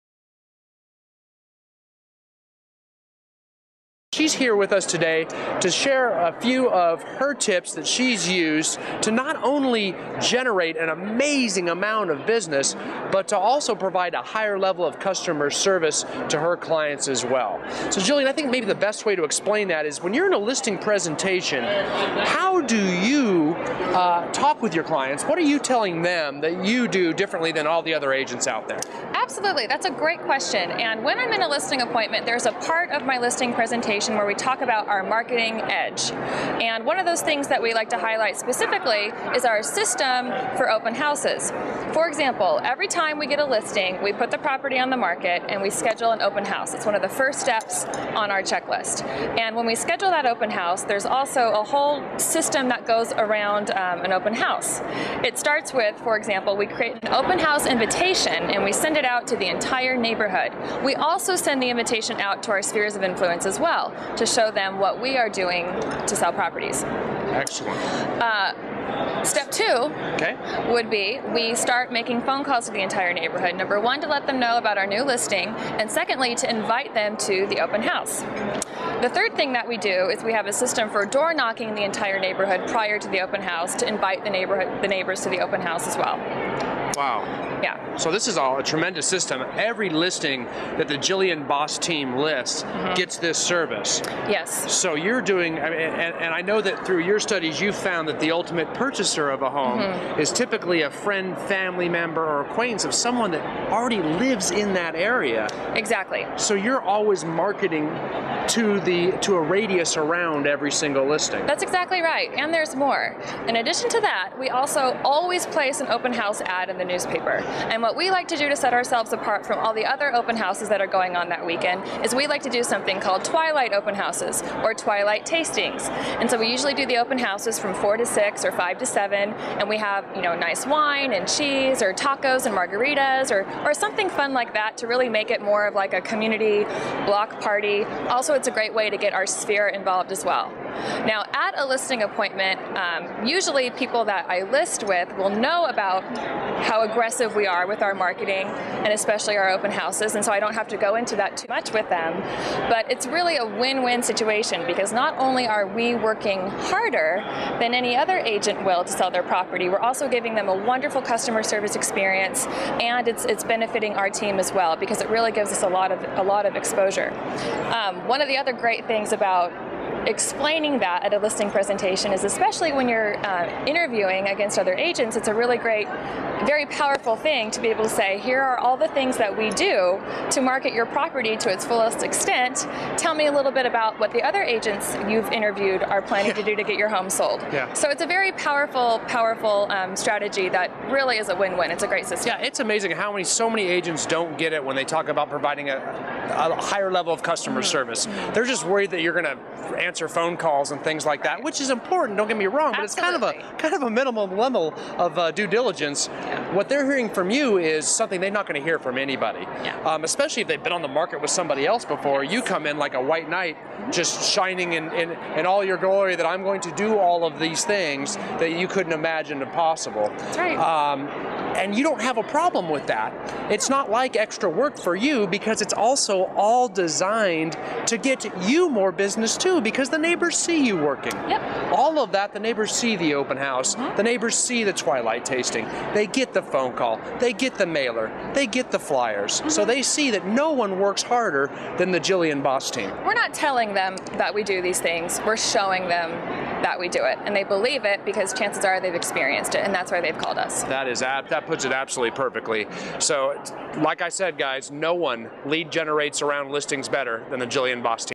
Generate New Listings From Existing Listings
- Mega Luxury Agent Jillian Bos explains her team's method for systematically marketing around existing listings to generate more business while also providing the highest level of customer service.